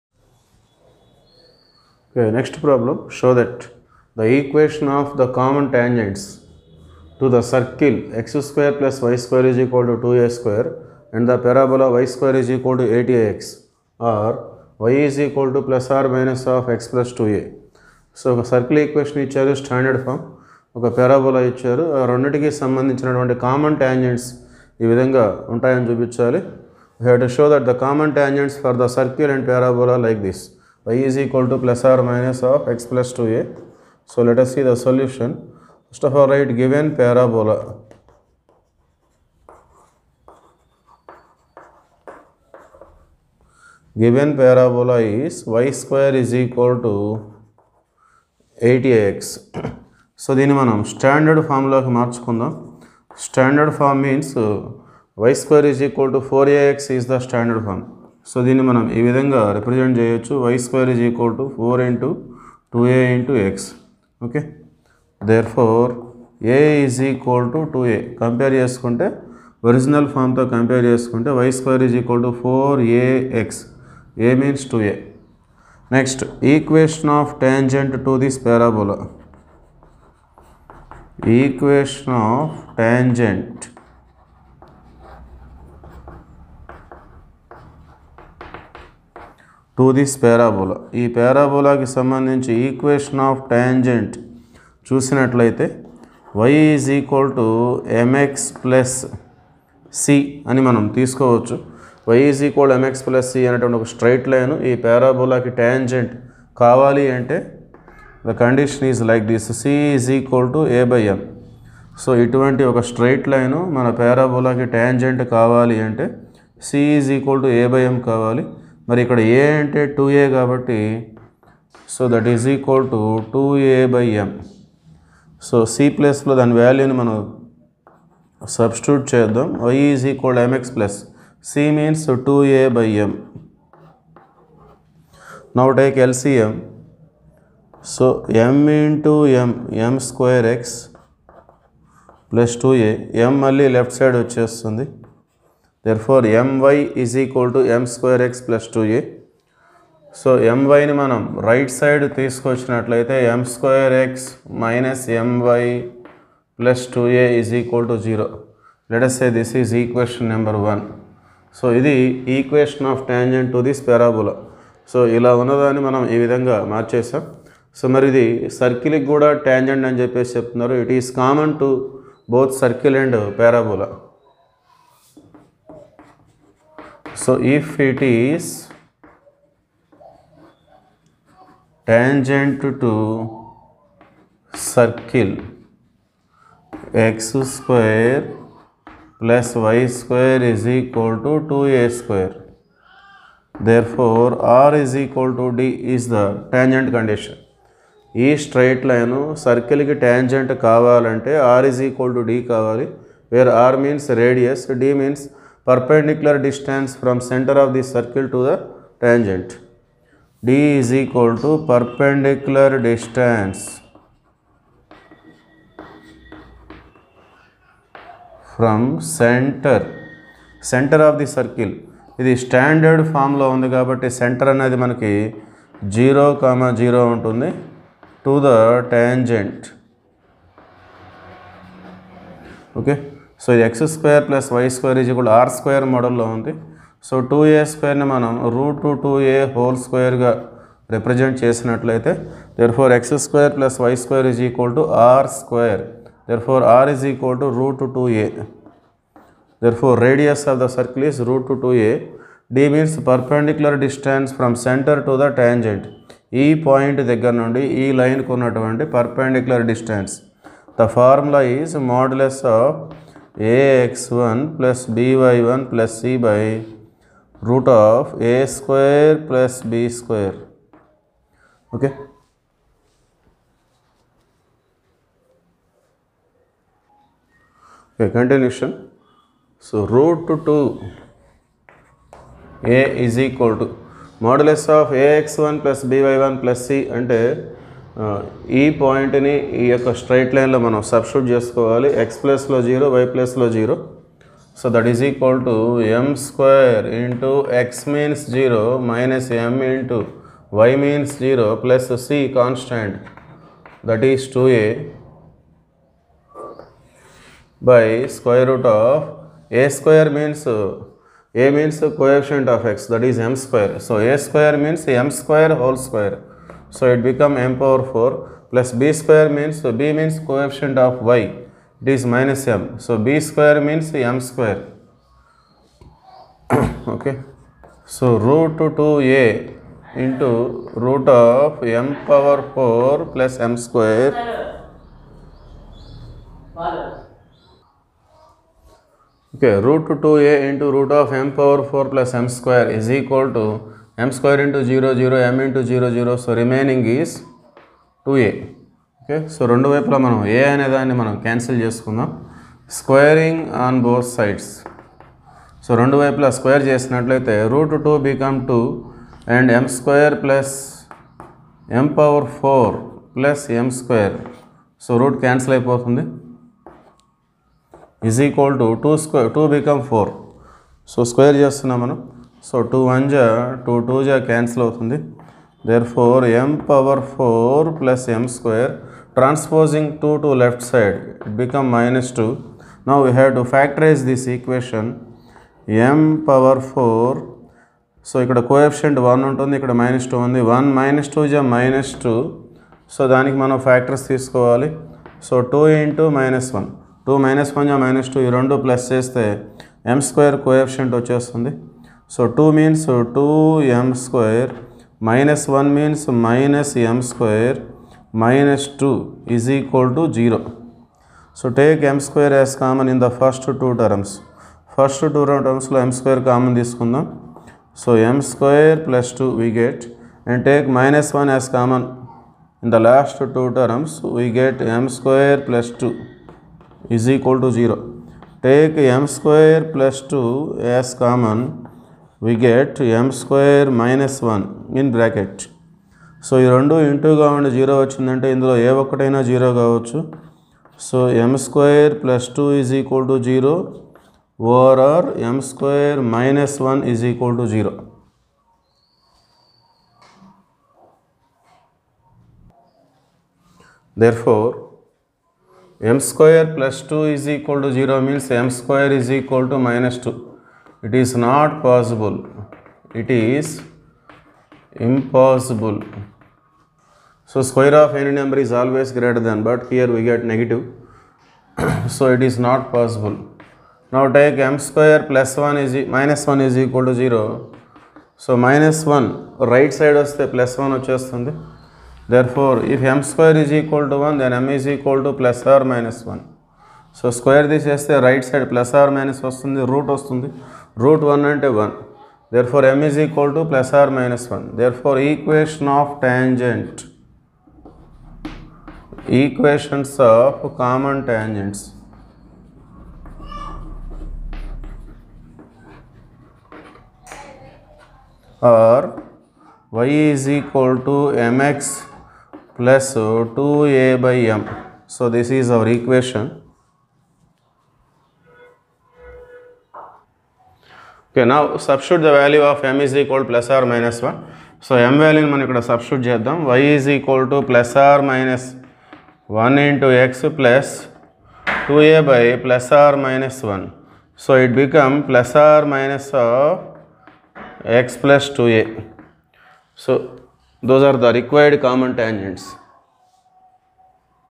Eq'n of common tangents to the circle x² + y² = 2a² & parabola y² = 8ax are y = ±(x + 2a)
Parabola | 7 marks | Equations of Common Tangents to Circle and Parabola @EAG